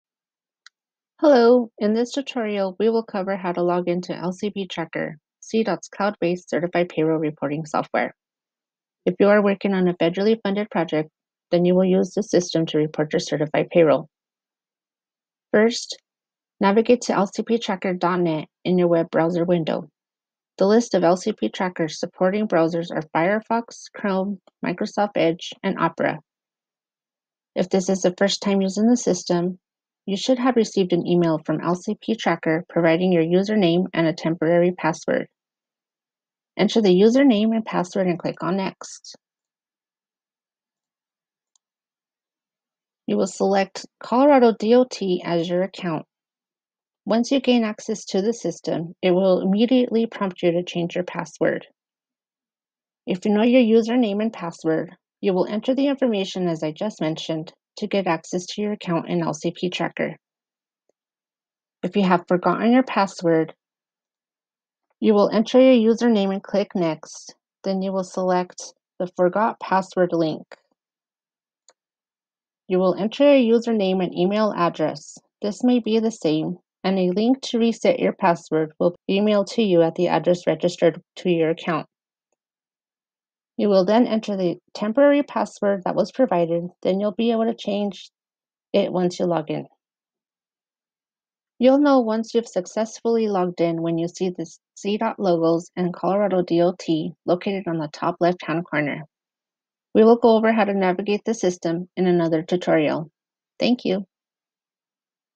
Logging into LCPtracker
updated April 2021

0:00 - Introduction
0:42 - Select COLORADO DOT account
1:01 - Forgot Password
1:31 - LCPtracker Dashboard